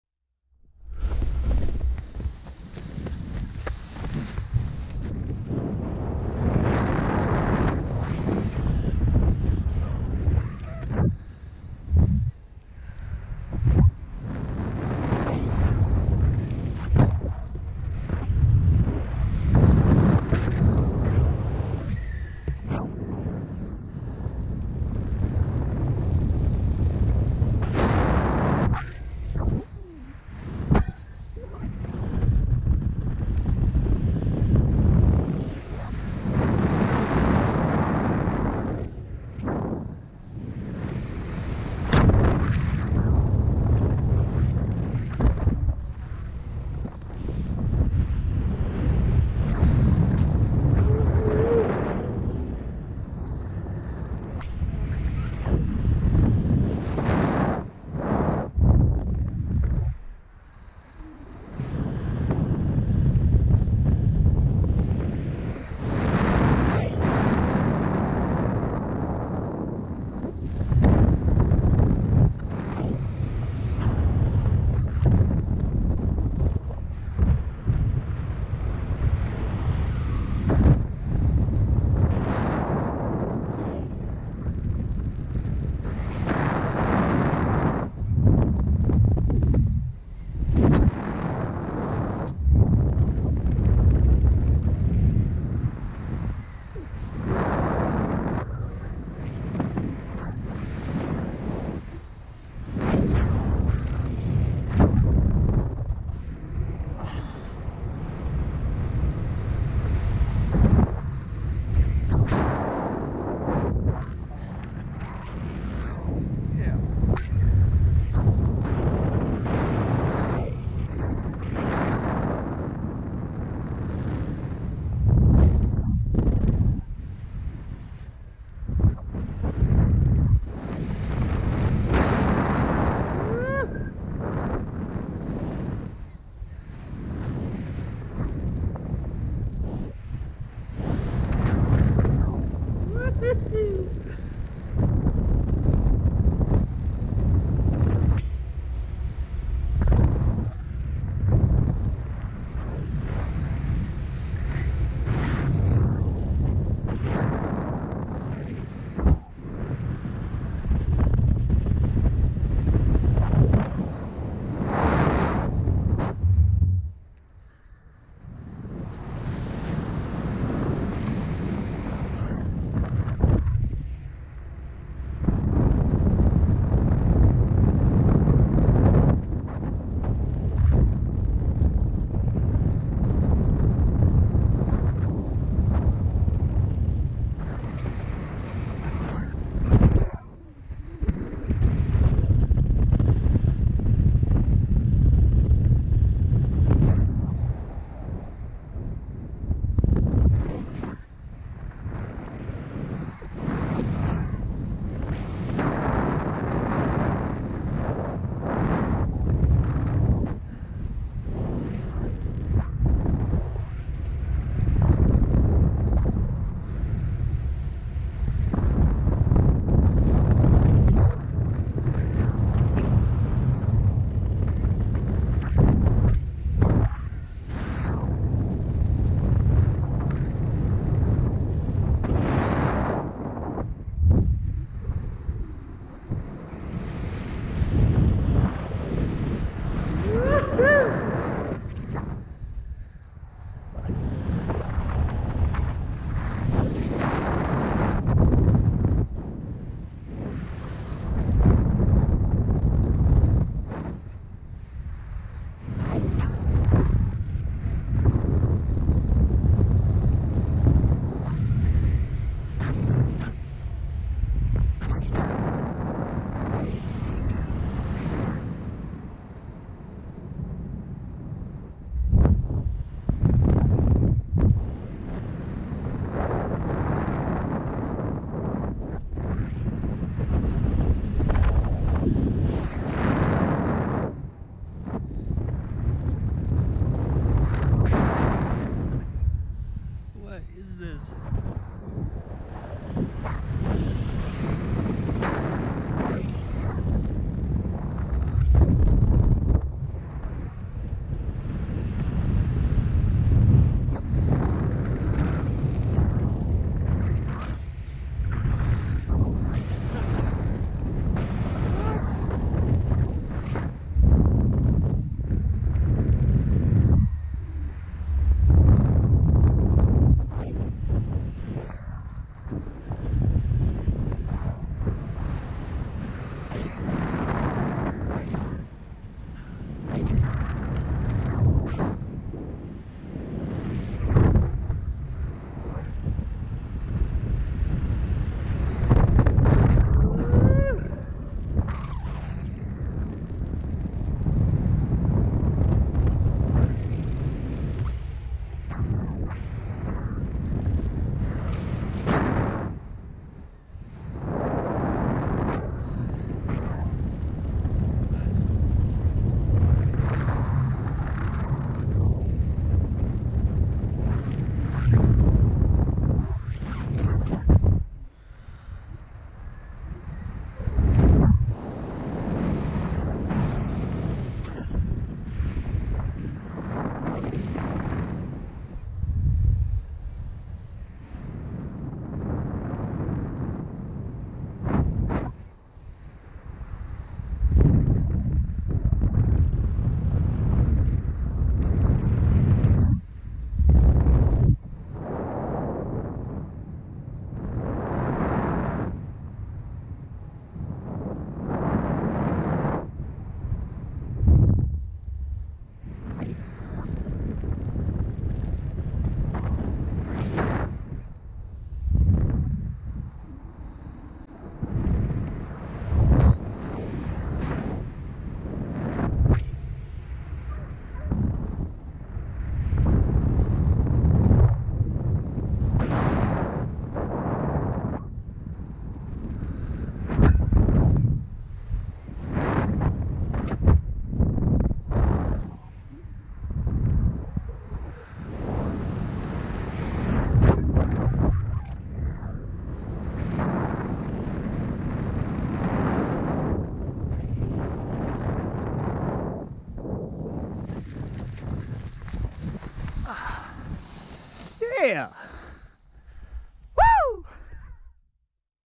Flight 41
Super fun flight from the Bozeman "M".

Ozone Geo 4, Gin SpeedRide, GoPro 6.